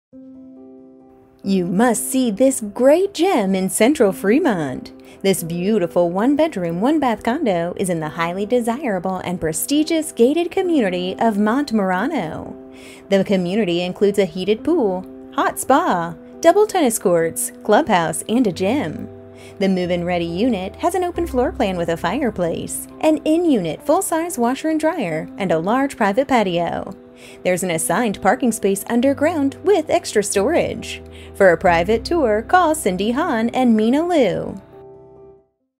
39059 Guardino Drive Unit #104 Fremont, CA | ColdwellBankerHomes.com
39059 Guardino Drive Unit #104, Fremont, CA

You must see this gem in central Fremont! This beautiful one bedroom, one bath condo is in the highly desirable and prestigious gated community of Monte Merano. The community provides some of the best amenities in Fremont including a well-maintained heated swimming pool, hot spa, double tennis court